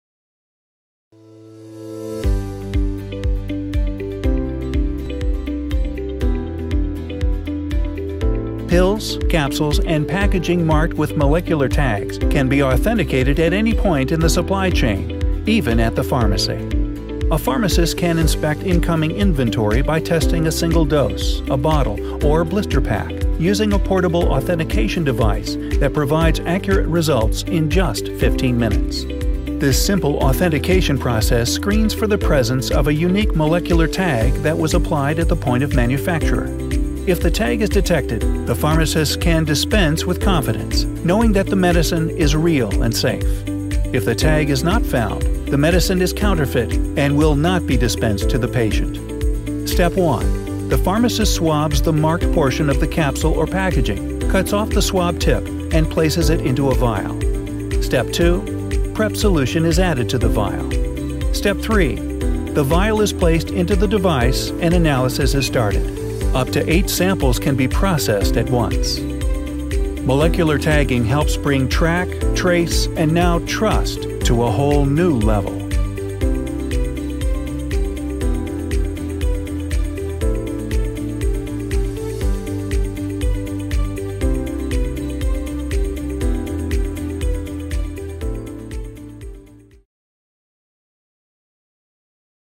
Authentic Pharmaceuticals — Trust is Only a Molecule Away™
DNA Molecular Tags Applied to Tablets Via Ink Through our partners, DNA Molecular Tags have been applied to pharmaceutical-grade, edible ink that would be utilized for printing on pharmaceutical capsules or tablets. DNA Molecular Tags Applied to Pharmaceutical Packaging Utilizing ink as a carrier, DNA Molecular Tags can be applied to the packaging of any pharmaceutical product, fully securing the package. Barcode labels can be printed with the DNA Molecular Tag, enabling complete track and trace.